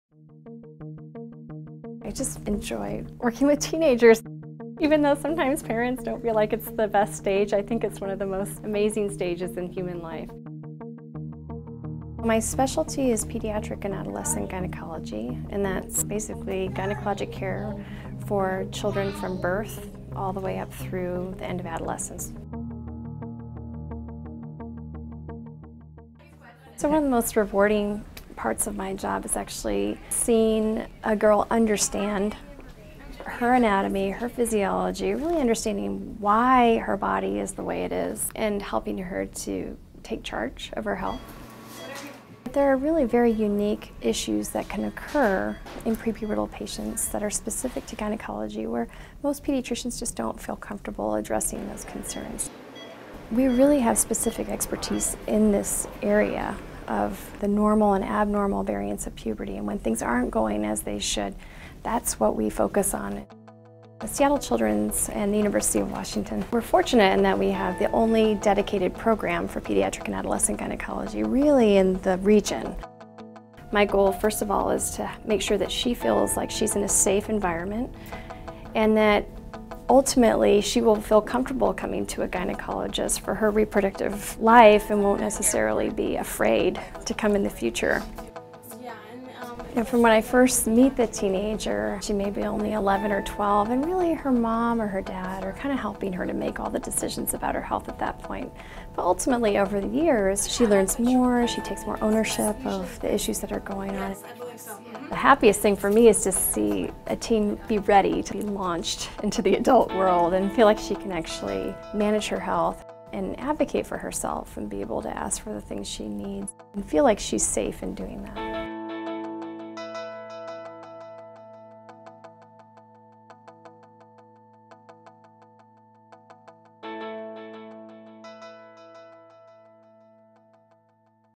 Meet Dr. Anne-Marie Amies Oelschlager
Dr. Anne-Marie Amies Oelschlager, pediatric and adolescent gynecologist at Seattle Children's Hospital and assistant professor of Obstetrics and Gynecology at the University of Washington School of Medicine discusses how she helps her patients make educated choices and optimally manage their own reproductive health.